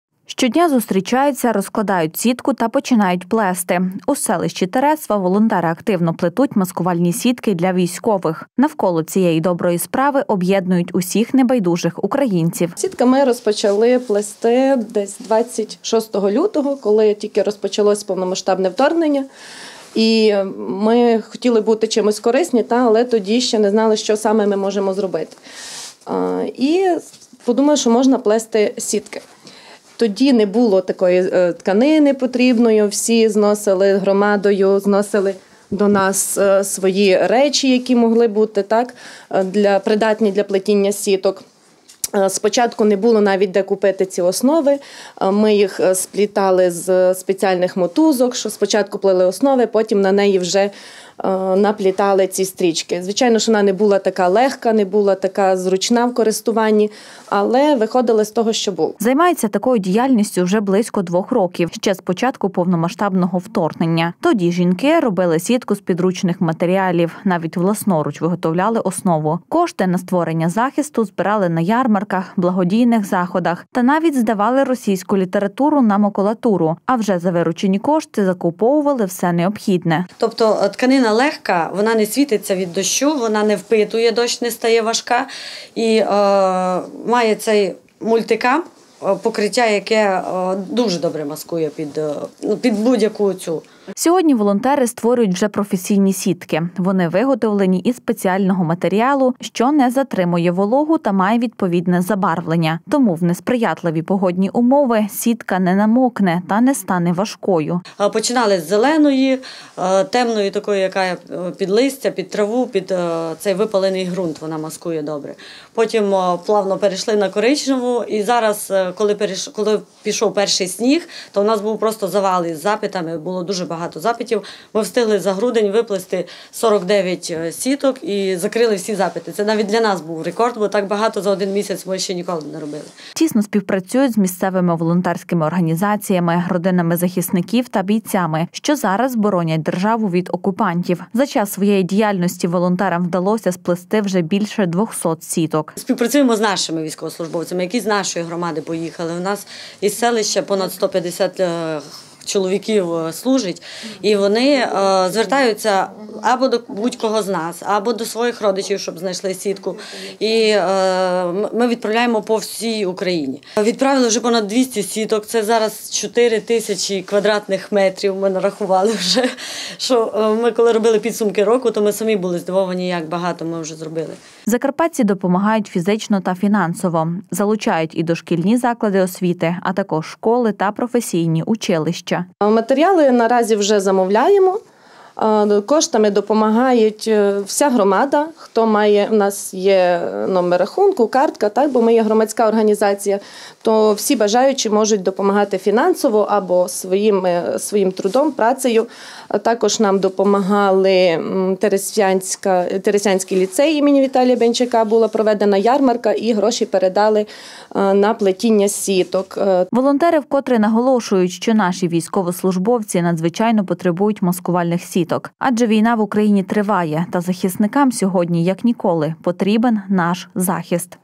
Плетуть захист
Щодня зустрічаються, розкладають сітку та починають плести. У селищі Тересва волонтери активно плетуть маскувальні сітки для військових. Навколо цієї доброї справи об’єднують всіх небайдужих українців.  Займаються такою діяльністю вже близько двох років, ще з початку повномасштабного вторгнення. Тоді жінки робили сітку з підручних матеріалів, навіть власноруч виготовляли основу. Кошти на створення захисту збирали на ярмарках, благодійних заходах, та навіть здавали російську літературу на макулатуру, а вже за виручені кошти закуповували все необхідне.  Сьогодні волонтери створюють вже професійні сітки. Вони виготовлені із спеціального матеріалу,  що не затримує вологу та має відповідне забарвлення. Тому у несприятливі погодні умови, сітка не намокне та не стане важкою. Тісно співпрацюють з місцевими волонтерськими організаціям, родинами захисників та бійцями, що зараз боронять державу від окупантів. За час своєї діяльності волонтерам вдалося сплести вже більше 200 сіток. Закарпатці допомагають фізично та фінансово.  Залучають і дошкільні заклади освіти, а також школи  та професійні училища.  Волонтери вкотре наголошують, що наші військовослужбовці  надзвичайно потребують маскувальних сіток. Адже війна в Україні триває та захисникам сьогодні як ніколи потрібен наш захист.